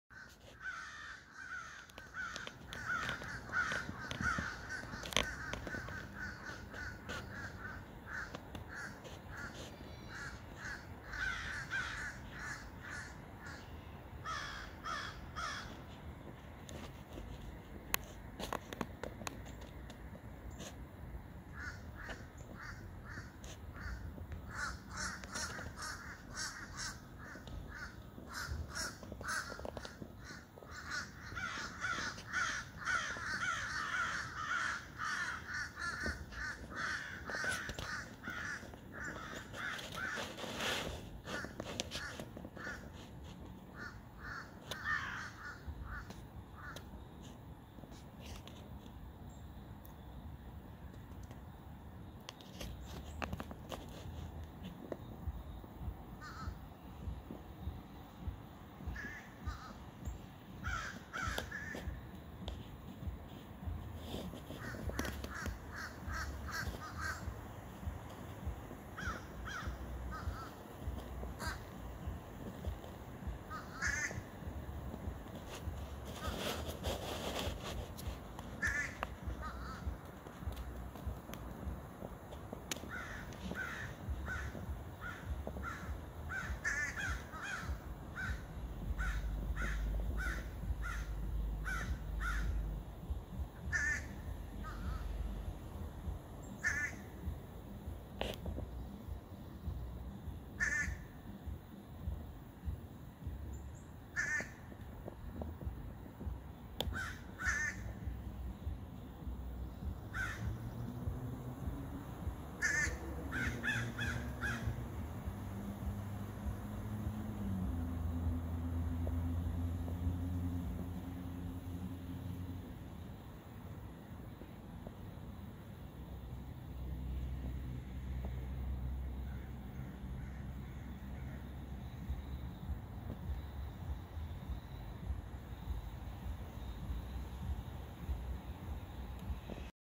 A Whole Crew of Crows Cawing Like Crazy!
cawwww!